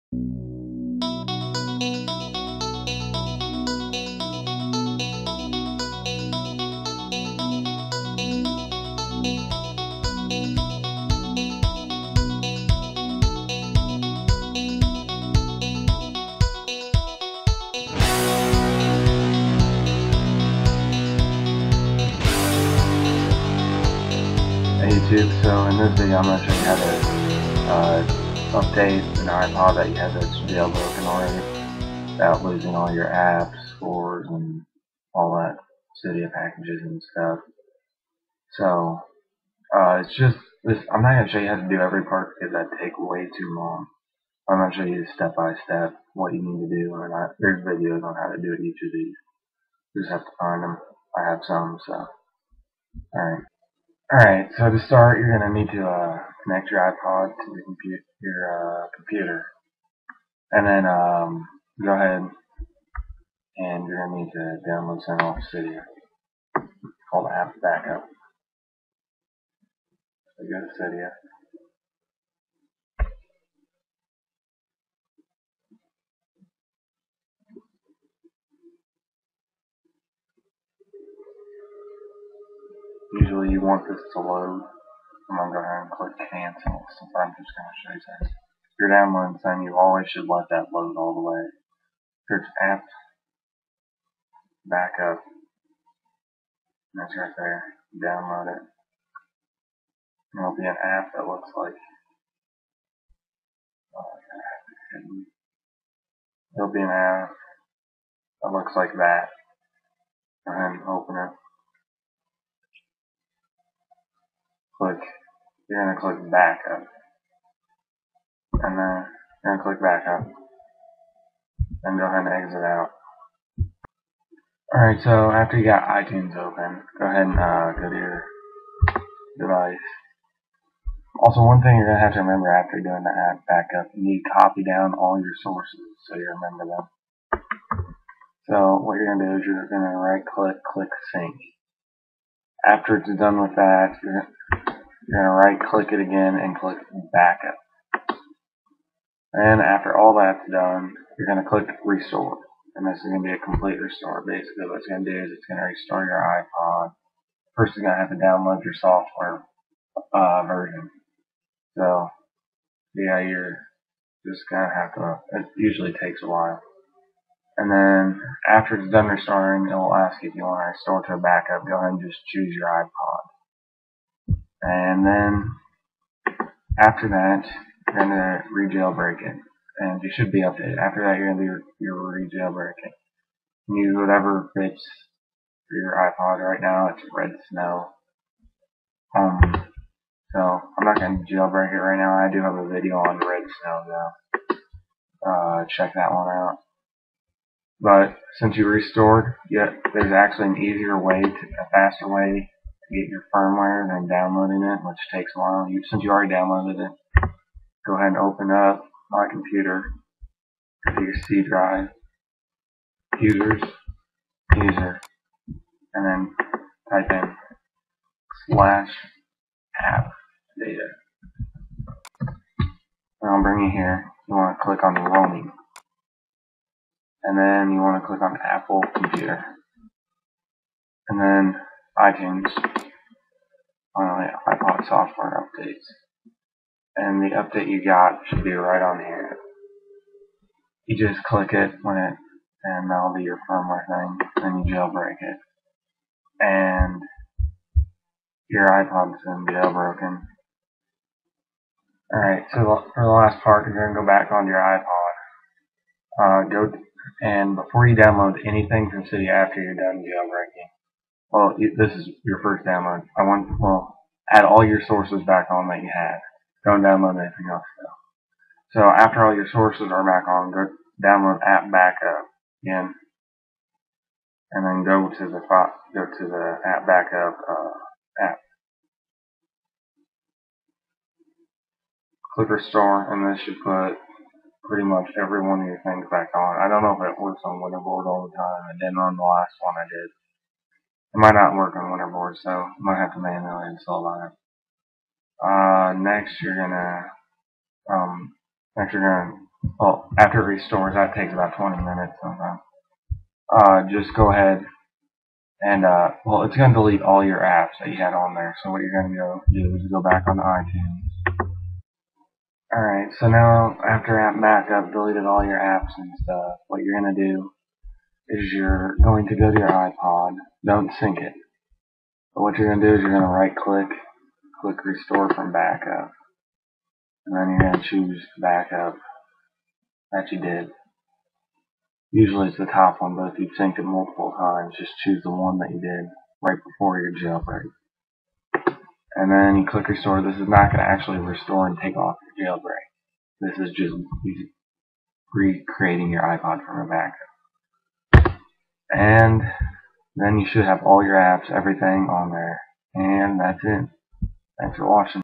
How To: Update a Jailbroken iPod, iPhone, or iPad
This shows you how to update a jailbroken ipod touch, iphone, or ipad while keeping all your packages and apps.